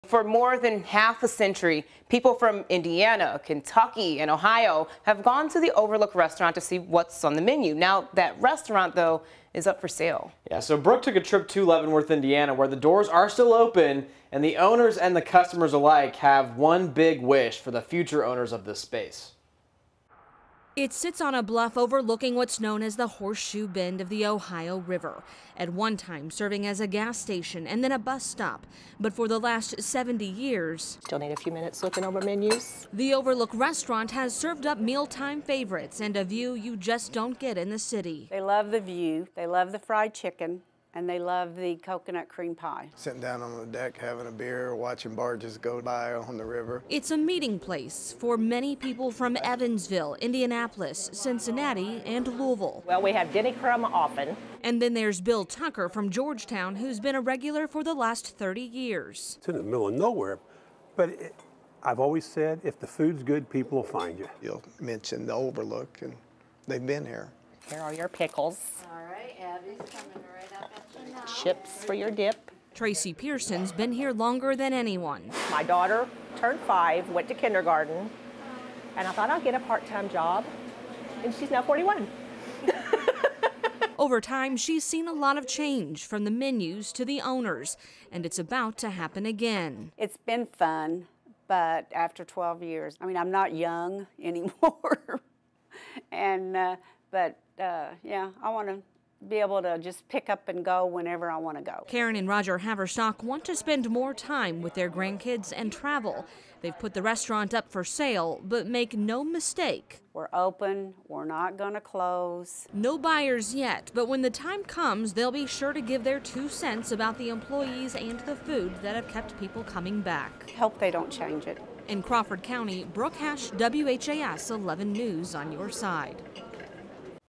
This beloved restaurant in Indiana is for sale, but still serving meals and great views
For the last 70 years, the Overlook Restaurant has served up mealtime favorites and a view you just don't get in the city. Locals hope that won't change.